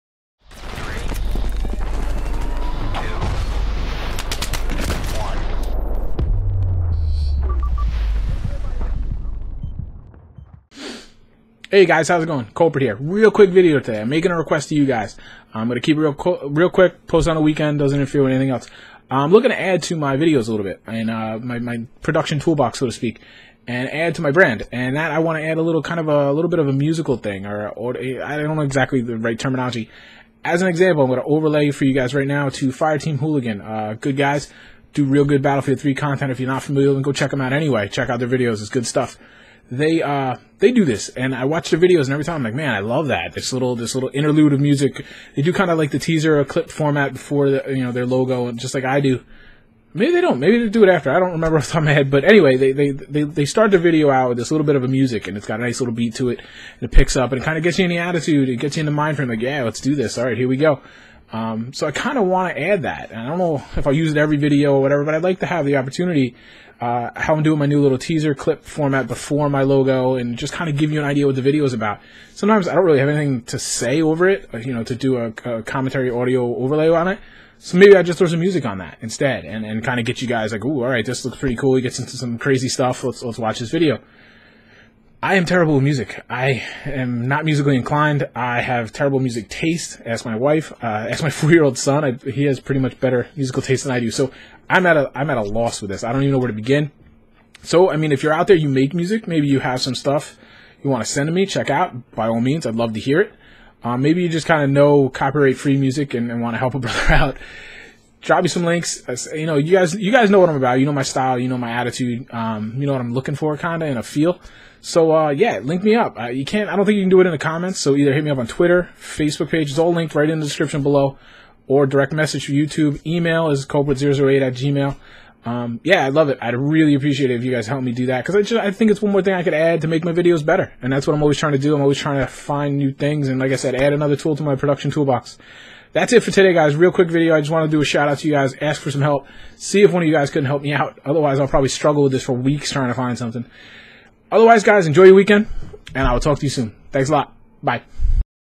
Good with music? I need your help!
This is a quick video asking you guys for some help with some music I want to add to my bag of tricks.

Xbox Tag: Kulprit008
PS3 SEN: Kulprit008
Origin: Kulprit008
Steam: Kulprit008